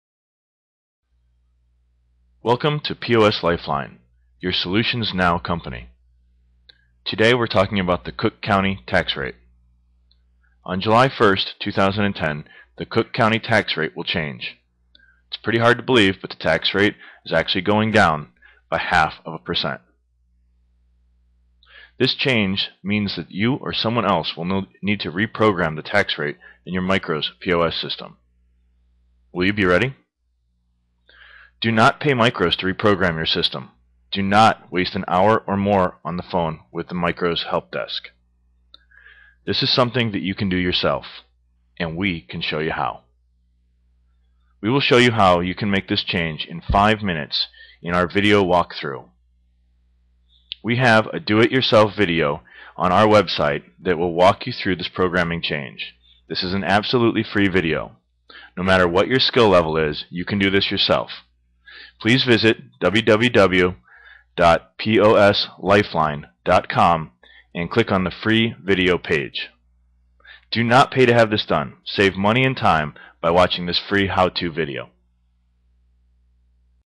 Tax Change Info.mp4
presents a video about the Cook County tax rate change and how you can program your Micros POS System for free! How to video on how to change the tax rate.